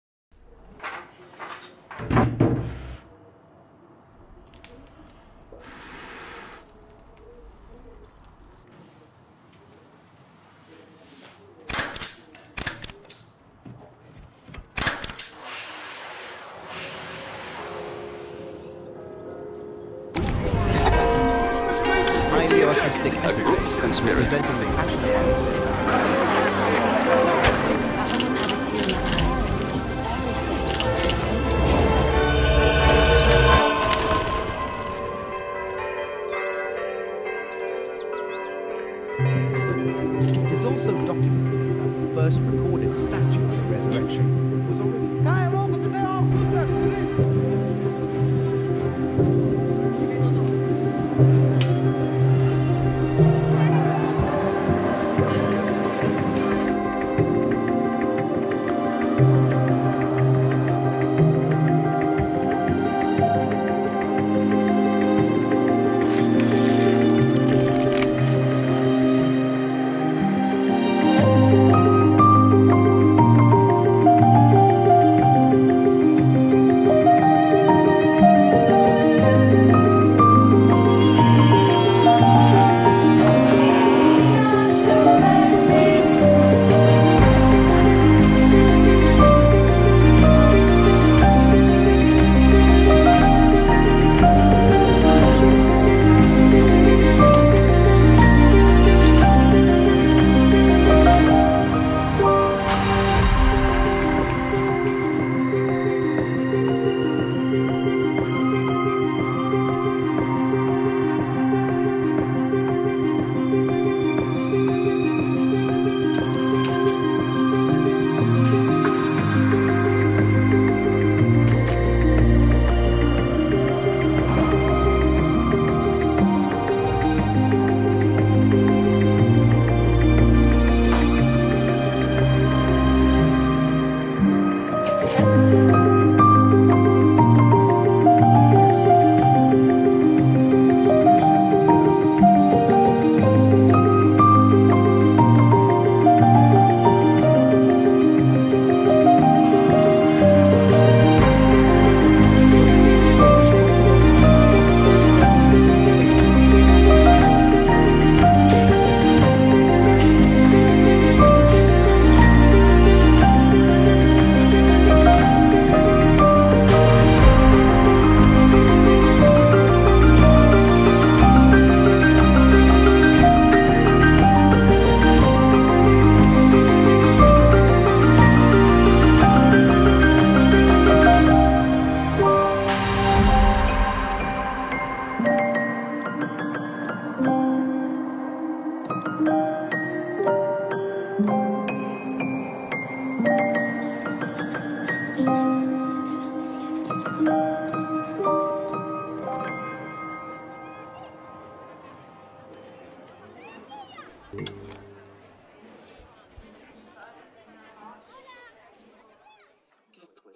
European Network of Holy Week and Easter Celebrations
The European Network of Holy Week and Easter Celebrations is a cultural route that is preparing its application to become a Cultural Route of the Council of Europe. It is an international network of tourist destinations that are famed for the way they fervently celebrate Holy Week and Easter each year.